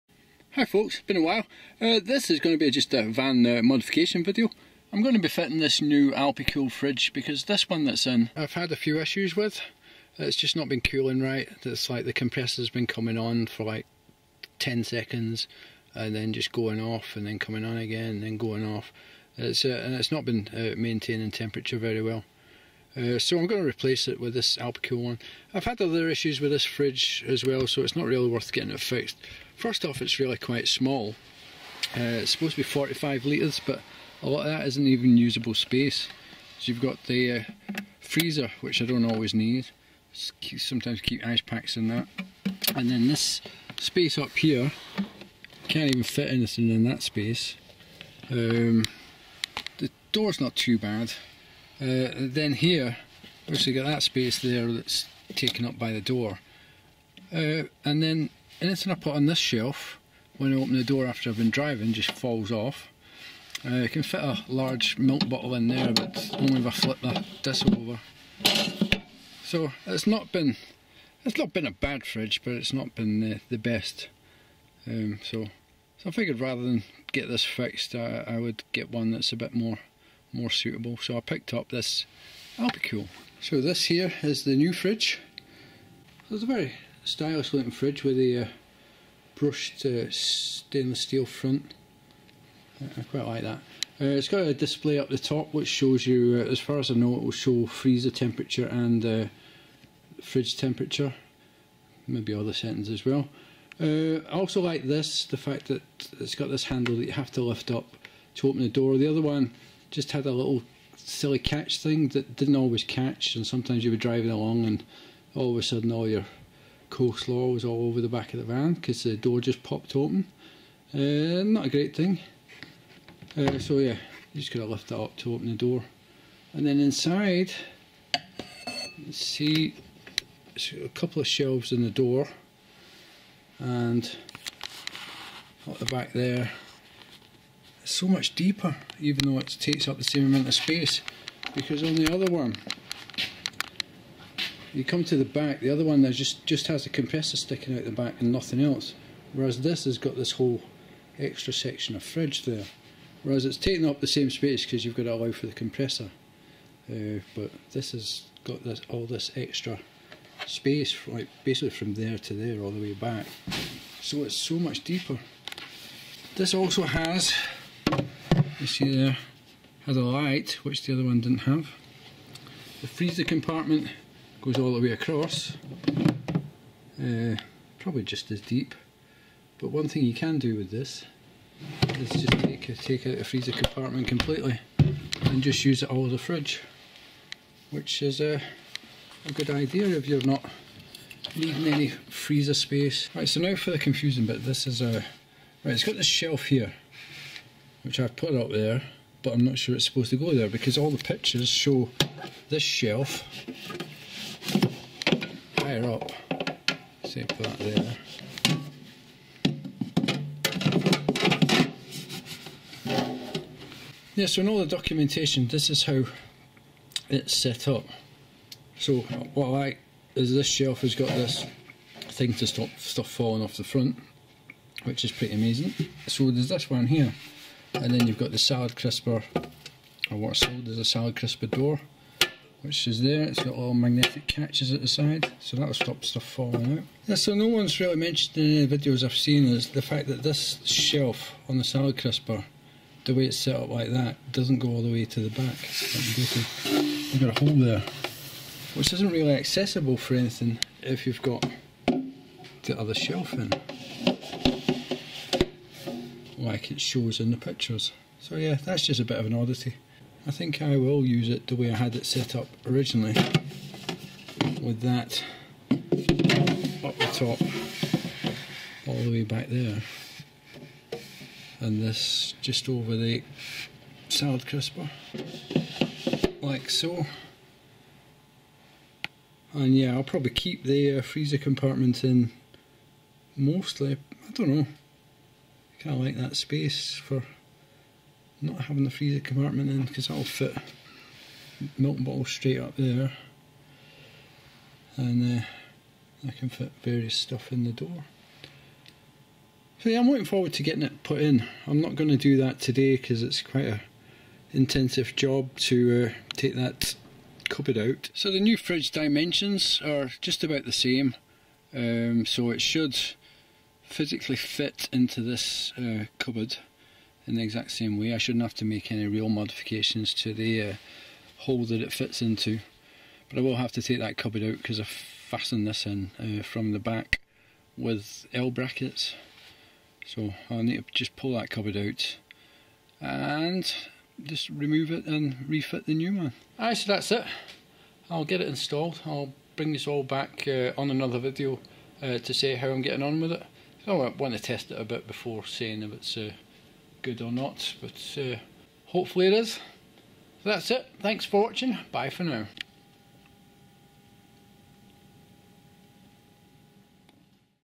Alpicool CR50X 12Volt Compressor Fridge First Look
Having put up with my campervan's existing IndelB compressor fridge playing up for far to long I decided to invest in a new Alpicool CR50X 12 Volt compressor Fridge. It's a nice looking unit and I'm looking forward to getting it fitted. This is the first look at it before installation.